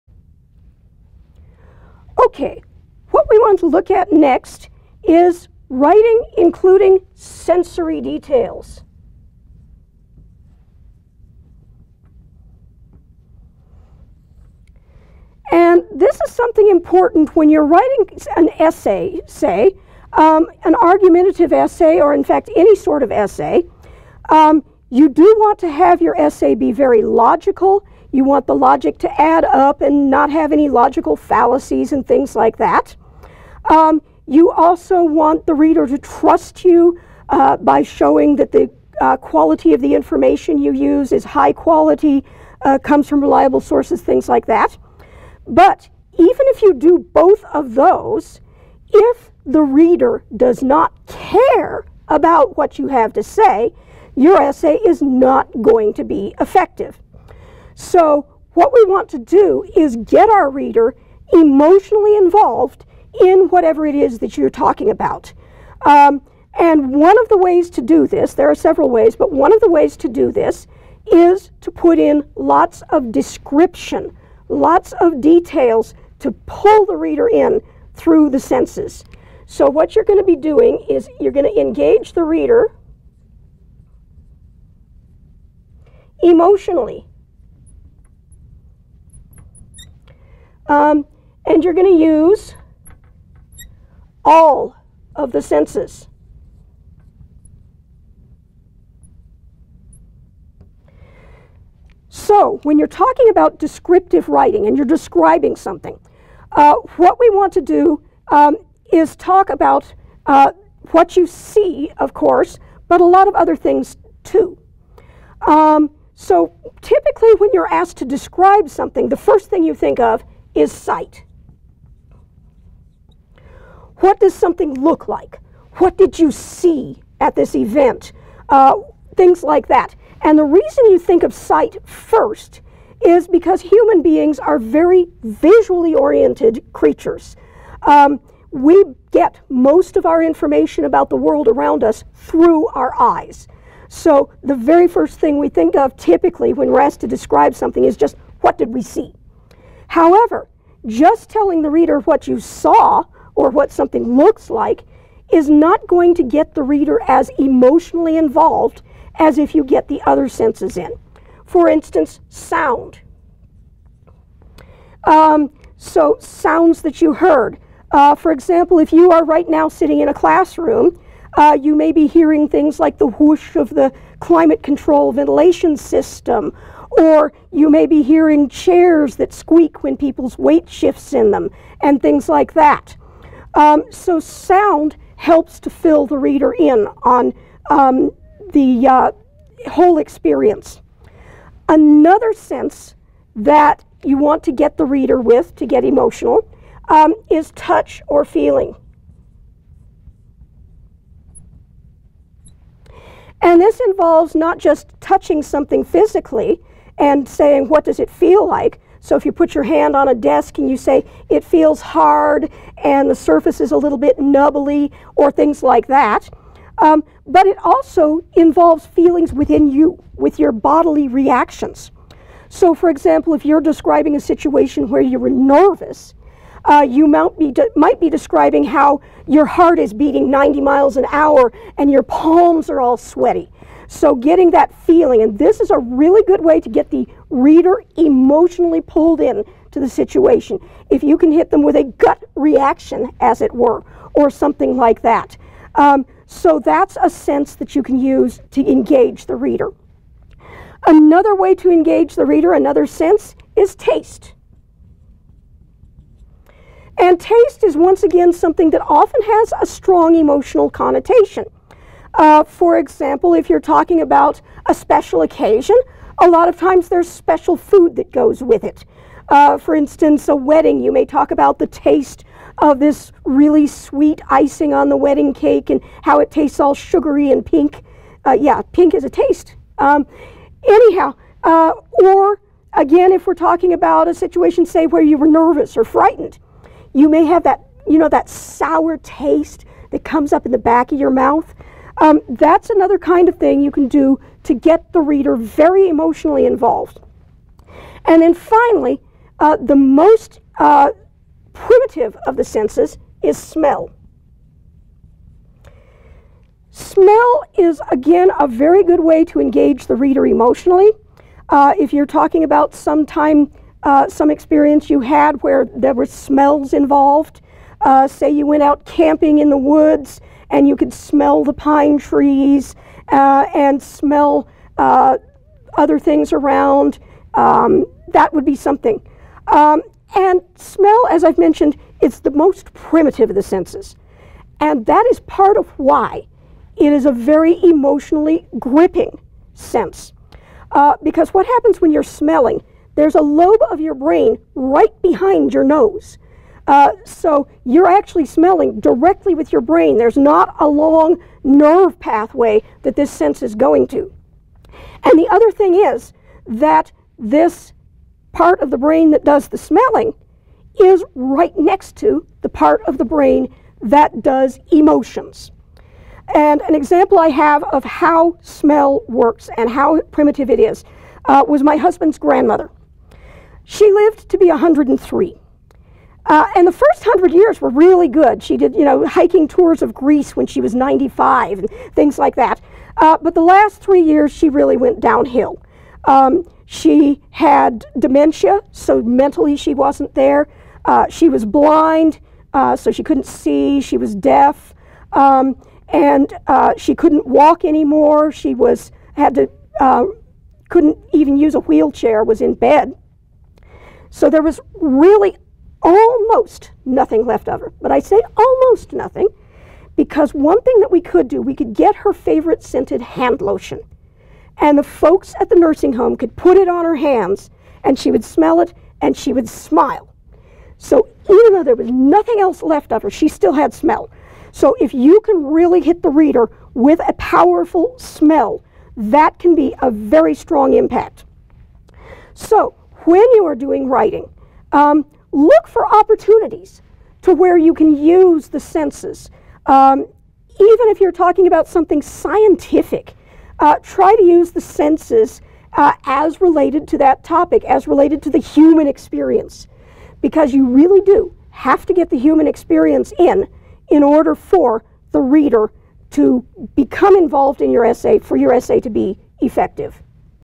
Sensory Details
When you write an essay, you can have the most solid logic in the world, and the most reliable information in the world, but if your reader doesn't care about what you have to say, your essay will not be effective. One way to grab the reader emotionally is by using sensory details to draw the reader into your topic.